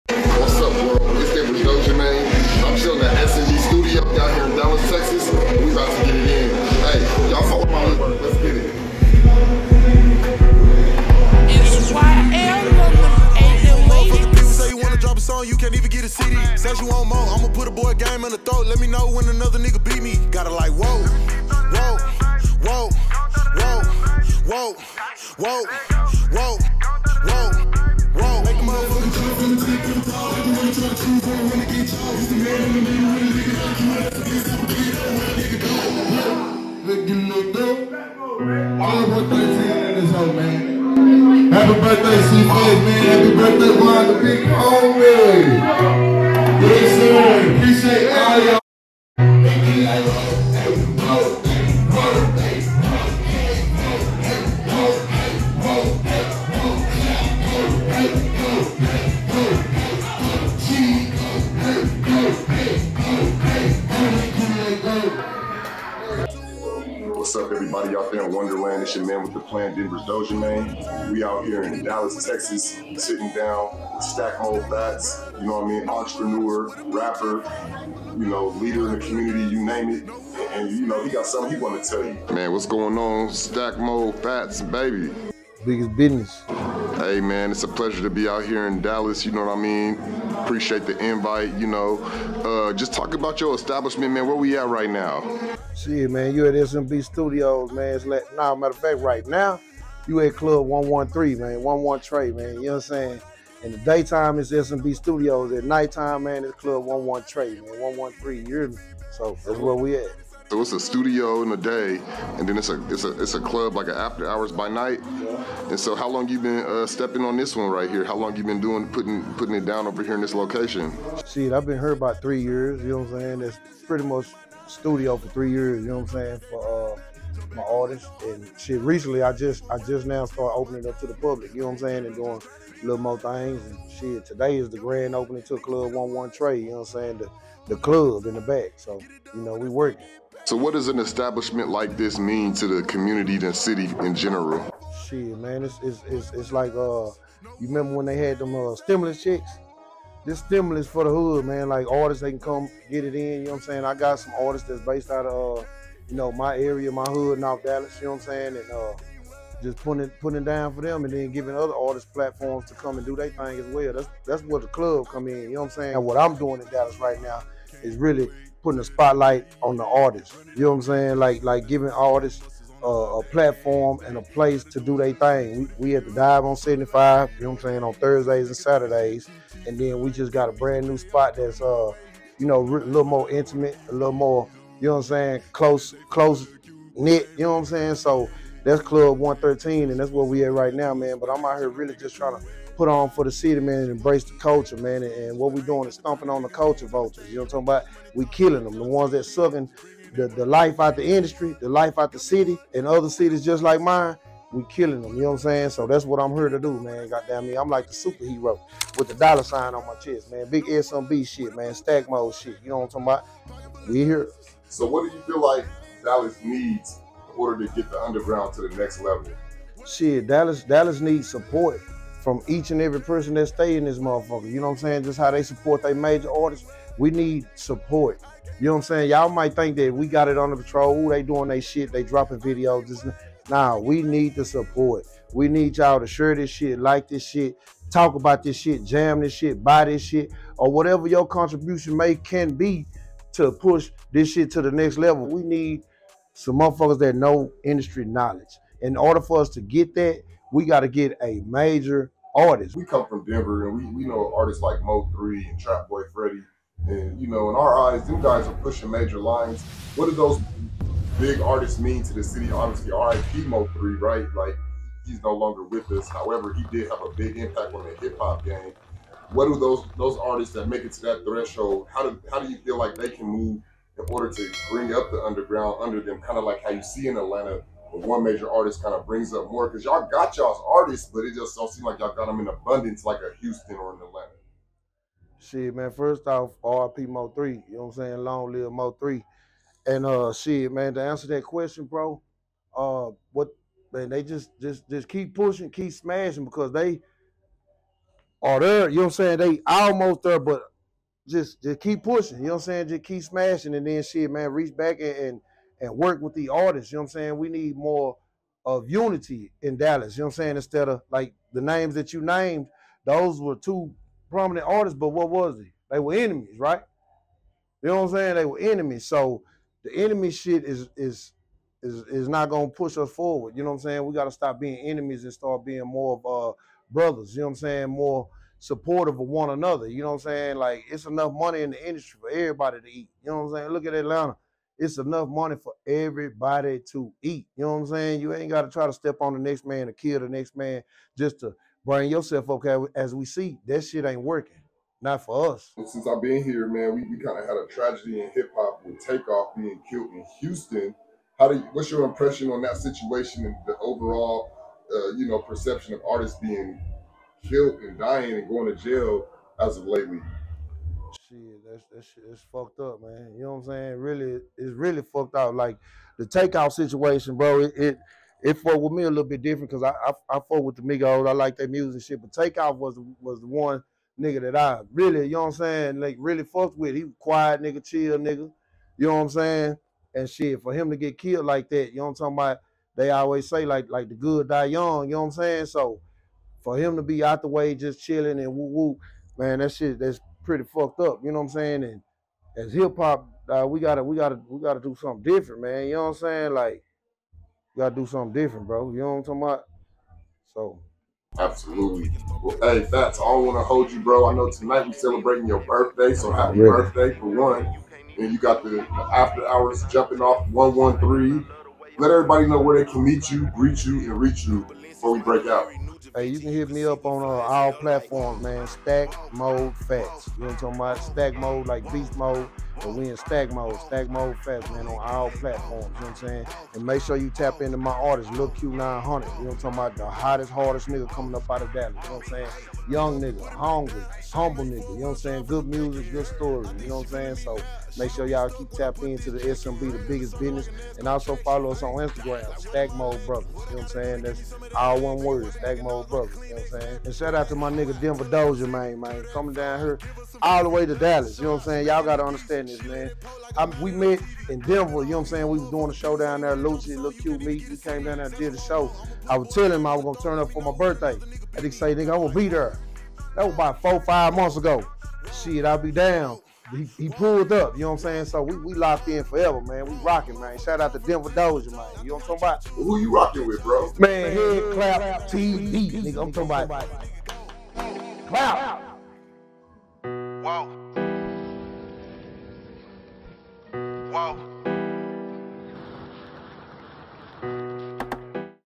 Head-Klap Entertainment ft. C-Fats, CEO of Stackmode Brothers.
On this exciting webisode of Head-Klap Teavee Dojamain heads South West to get up with SMB CEO STACK MODE FATS to discuss the roll he plays in spotlighting the Dallas Underground Hip Hop scene, the current condition of Artist from the city gaining more exposure, the recent passing of Take Off and more...So Sit Tight Letit Rocc It's Head-Klap Teavee Keep it Loqt🔒